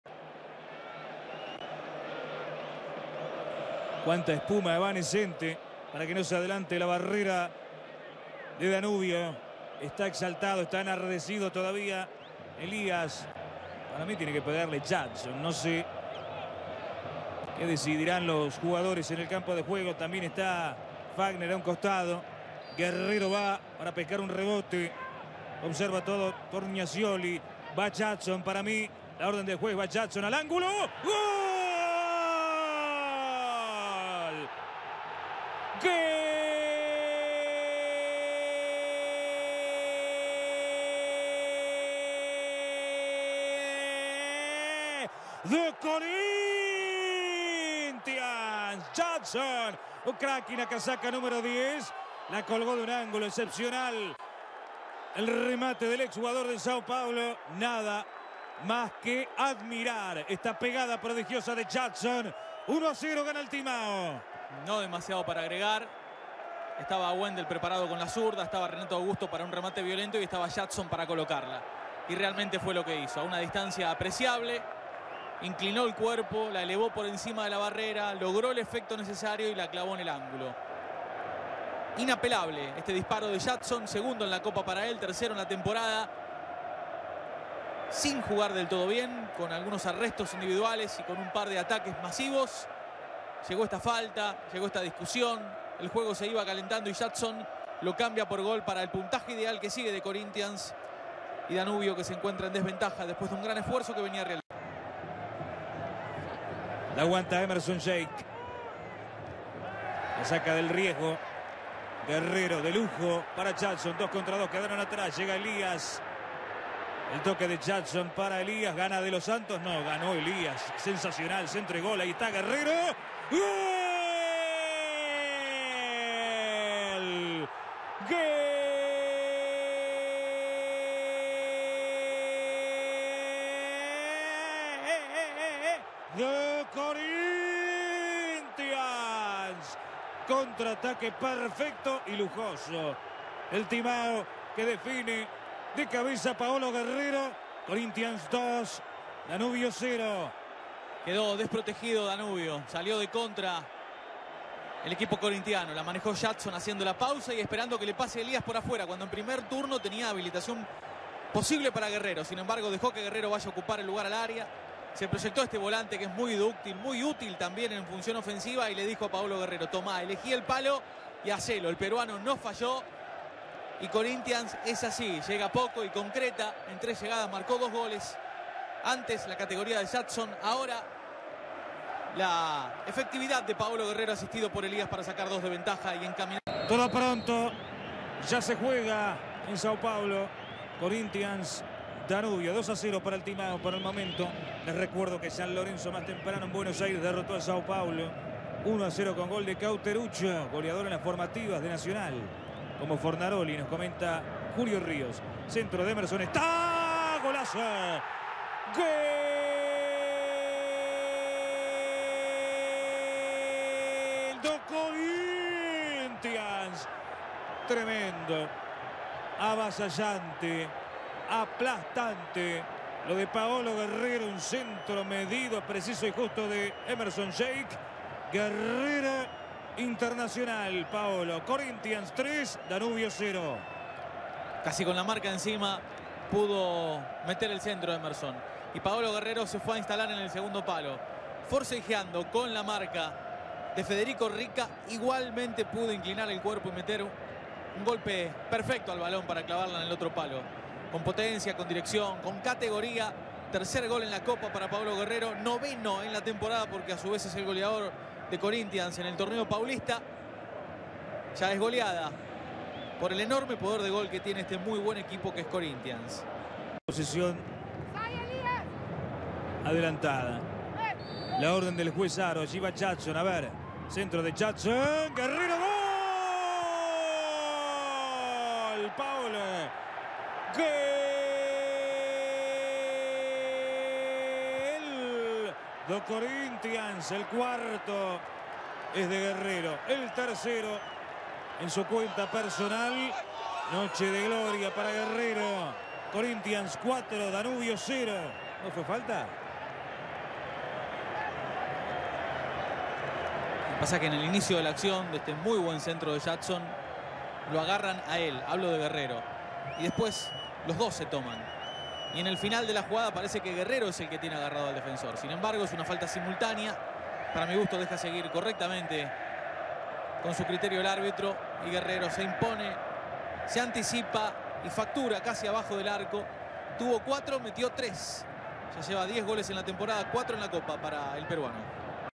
Corinthians 4 - 0 Danubio Copa Libertadores 2015
Copa Libertadores. Grupo 2, cuarta jornada.

Corinthians (BRA) 4 - 0 Danubio (URU).

Goles: 1-0, m.29: Jádson. 2-0, m.33: Paolo Guerrero. 3-0, m.46: Paolo Guerrero. 4-0, m.67: Paolo Guerrero.

- Ficha técnica:

4. Corinthians: Cássio; Fágner, Felipe (m.73, Edu Dracena), Gil, Uendel; Ralf, Elias (m.79, Petros), Renato Augusto, Jádson; Emerson Sheik (m.86, Vágner Love) y Paolo Guerrero. Entrenador: Tite.

0. Danubio: Franco Torgnascioli; Matías Velázquez (m.63, Jorge Graví), Cristian González, Matías De Los Santos, Federico Ricca; Agustín Viana (m.50, Santiago Schirone), Fabricio Formiliano, Nicolás Milesi (m.46, Ignacio González), Leandro Sosa; Bruno Fornaroli y Matías Castro. Entrenador: Leonardo Ramos.

Árbitro: el peruano Diego Haro expulsó a Matías De Los Santos por doble amonestación, y le mostró amarillas a Leandro Sosa, Cristian González y Jorge Graví, de Danubio, y a Emerson Sheik, de Corinthians.

Incidencias: partido por la cuarta jornada del Grupo 2 de la Copa Libertadores disputado en el estadio Arena Corinthians, de São Paulo, ante cerca de 38.471 asistentes.